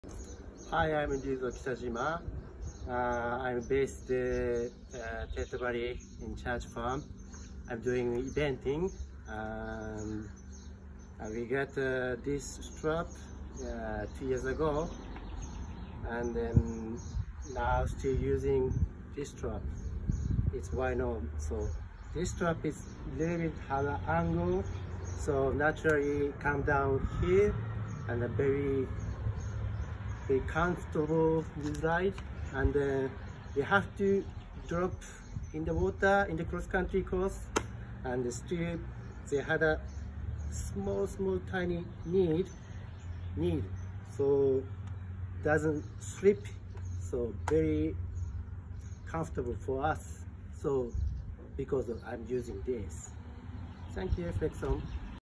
🎬thankyouflexon🎬Let’s meet our top ambassador from🇯🇵 Ryuzo Kitajima speaks about flex-on stirrups.
Green Composite 2 combines comfort and performance.
It provides a good stability for and perfect position of the leg.
While providing improved technical performance in terms of shock absorption and ergonomics,
Features :
• Green composite stirrup weight : 800 gr • Spring steel frame :
- Hot forged and treated against oxidation for strength and longevity.
- Overmolded with an environmentally-sourced polyamide material. • Interchangeable kits :
- footbed inserts : standard grip or ultra-grip (adhesion +)
- Magnetic stickers
• Footbed inclined, flat or endurance
• Offset slot for the stirrups leather (encourages optimum leg position
for better connection) • Can be customised